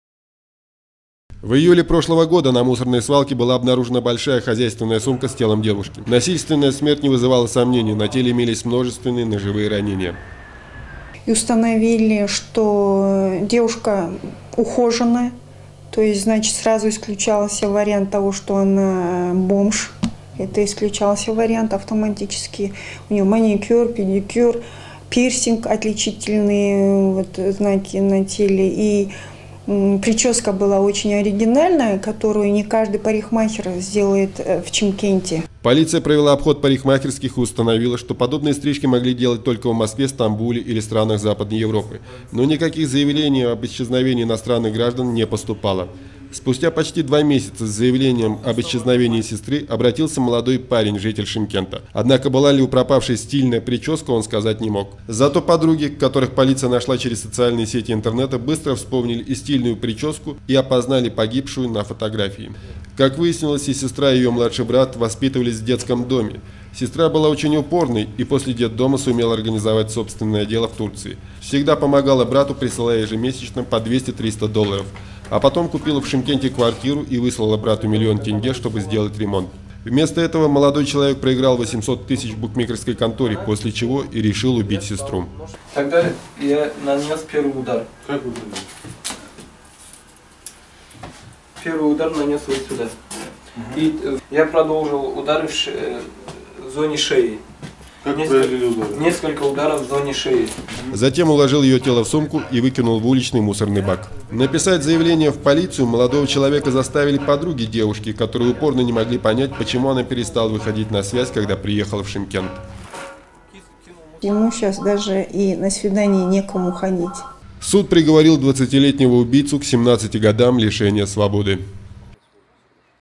Брат убил родную сестру из-за денег
Заявление в полицию молодого человека заставили написать подруги девушки, которые не могли понять, почему она перестала выходить на связь, когда приехала в Шымкент.
Суд приговорил 19-летнего убийцу родной сестры к 17-ти годам лишения свободы. Сейчас даже на свидание к парню прийти некому.